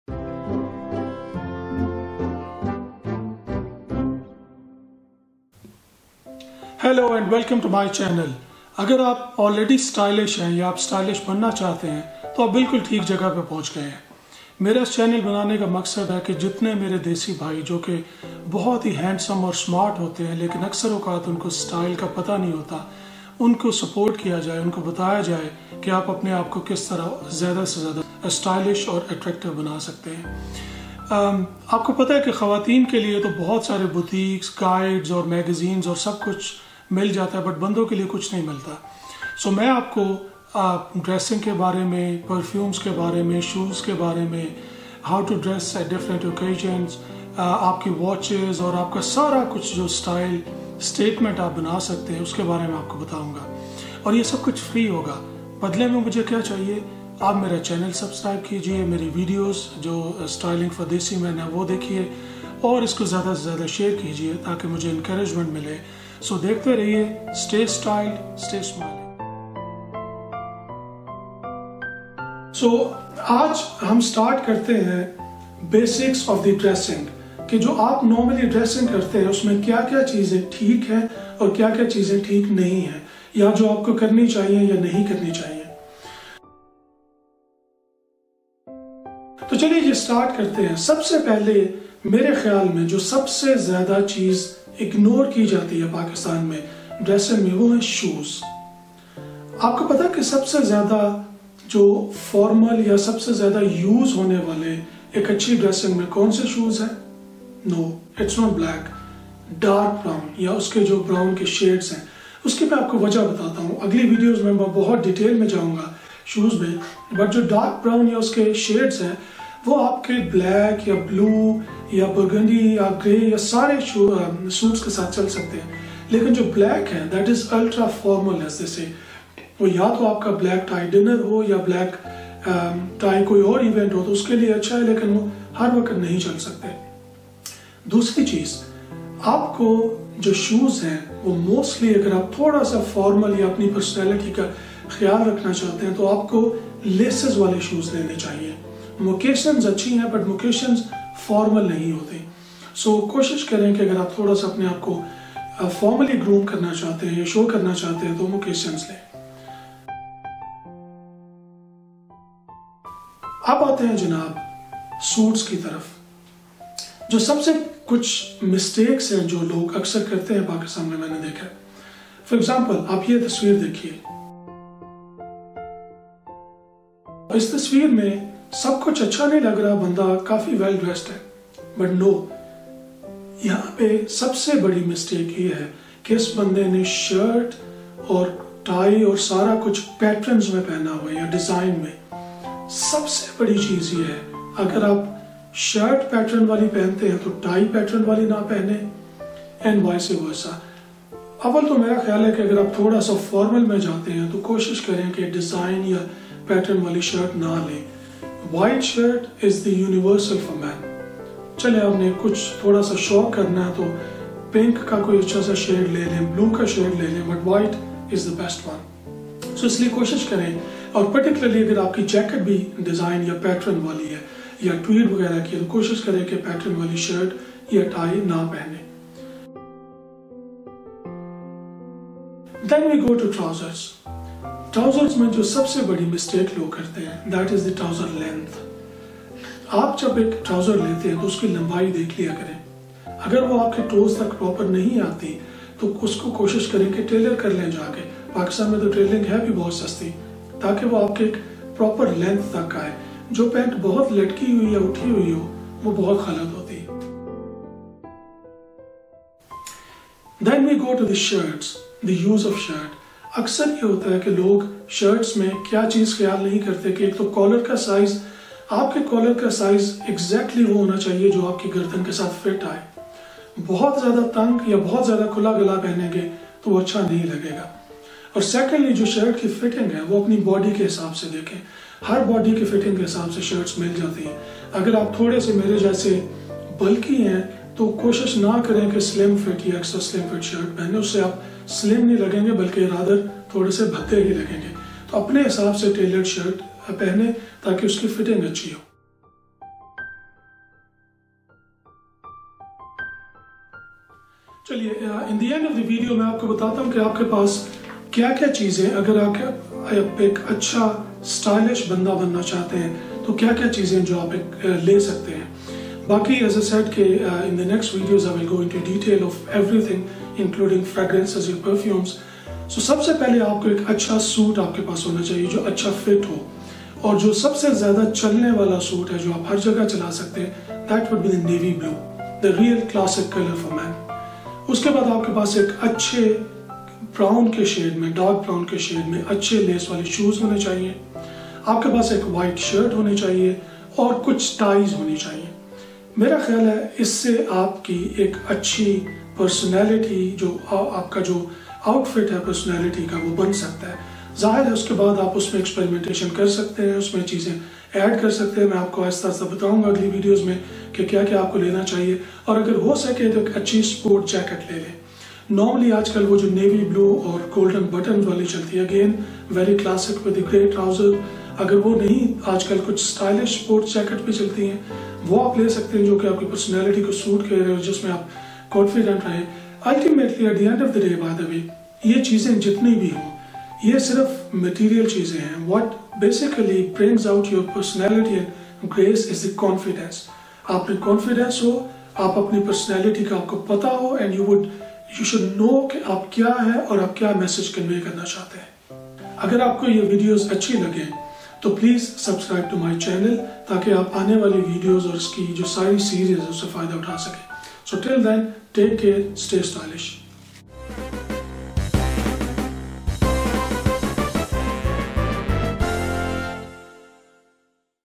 Basics of Men Dressing:- De-Style Guru [EP-1]
de-styleguru
Ahmad Mukhtar sharing some tips on how to up your style and take care of basic things in dressing.
In the next videos you will find details on different aspects, such as suits, shoes, perfume, watches and accessories.
If you wish to see some specific topic or issue, please write in comments section.
Stay Styled, Stay Smiling. Ahmad